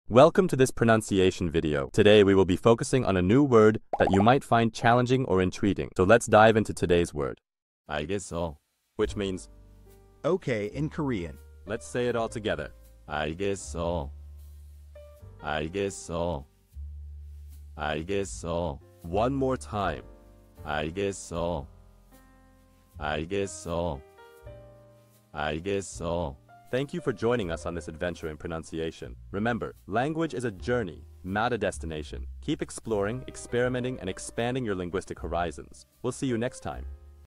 How to pronounce algesseo | 알겠어 (okay in Korean)
Master the Pronunciation of 'algesseo | 알겠어' - which means : okay in Korean 📝 with @PronunciationKorean 🎓🗣️ - [Your Guide to Korean]

Learn to pronounce 'algesseo' perfectly in Korean with our easy and engaging tutorial by @PronunciationKorean! 📖📽️ In this comprehensive video, we provide you with native speaker examples 🗣️👂 and phonetic breakdowns 🎵 to ensure you get the pronunciation just right.